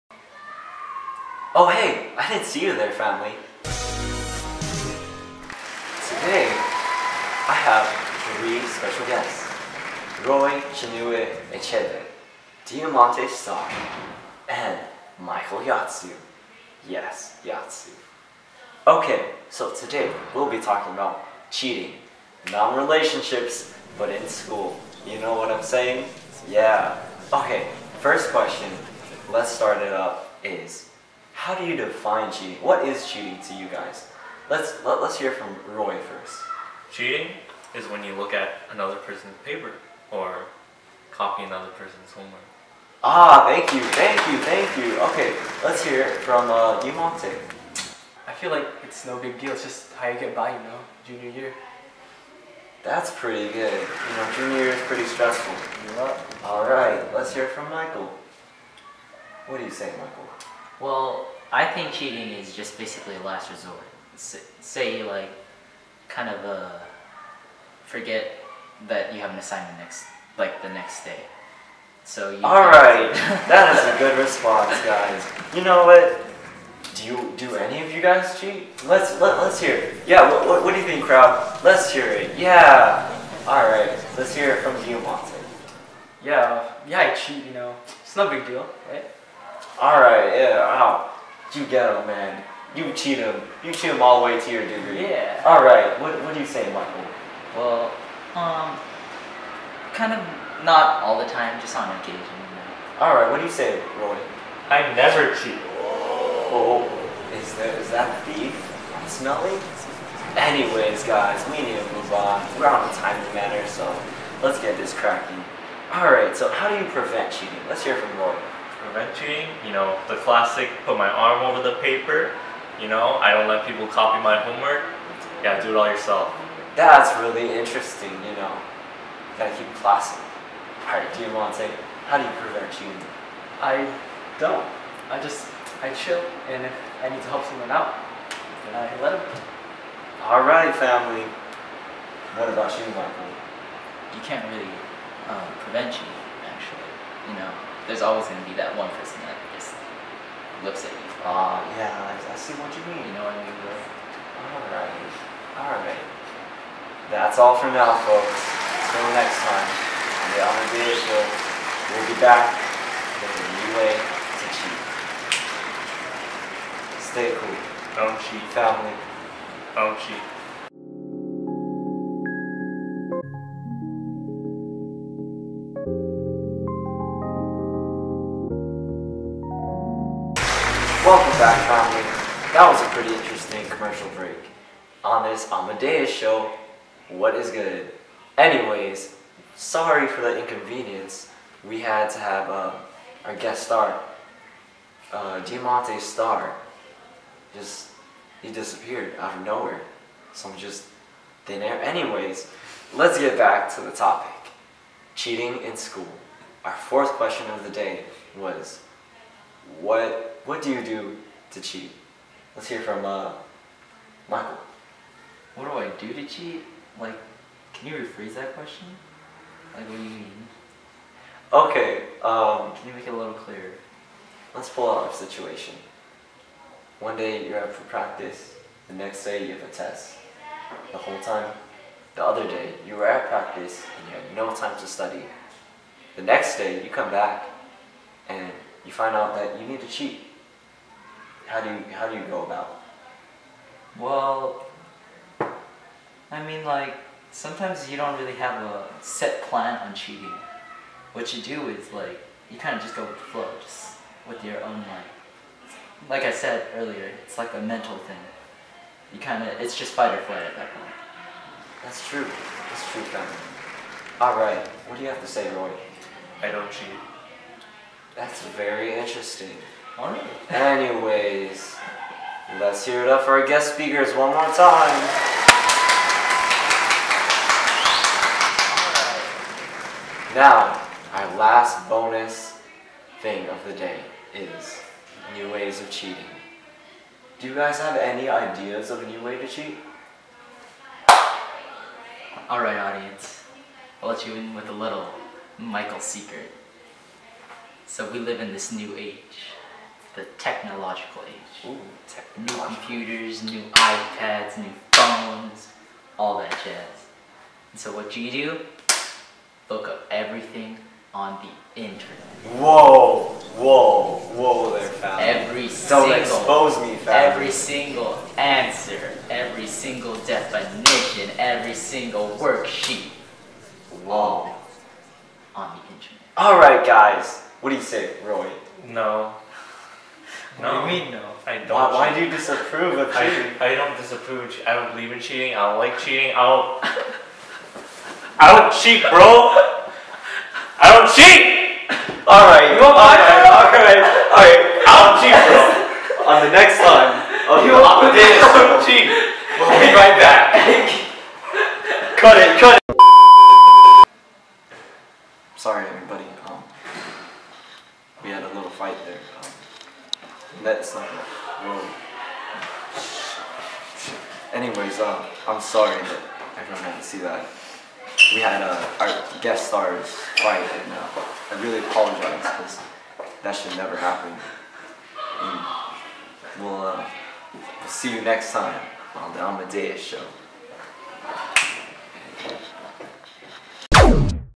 Amadaus Show: Cheating (Video Project)
Psychology Video Project for my boy Kobe
Update: He got a good grade